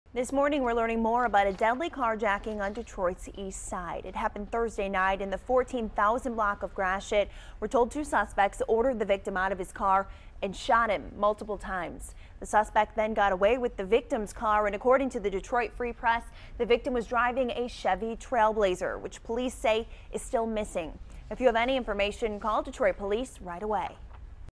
Detroit police search for suspects in deadly carjacking on Christmas eve
Detroit police are searching for suspects in a deadly carjacking of a man on Christmas Eve